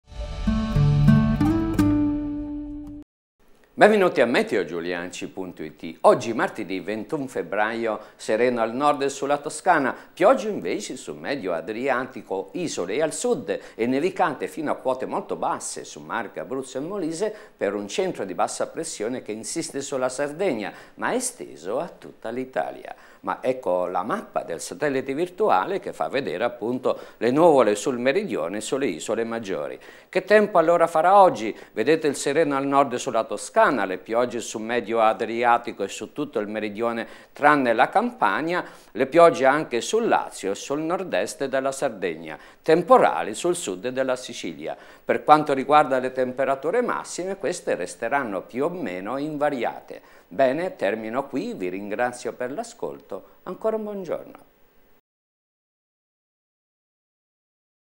Meteo di oggi, martedì 21 febbraio 2012.
Le previsioni del Colonnello Mario Giuliacci per oggi, martedì 21 febbraio 2012.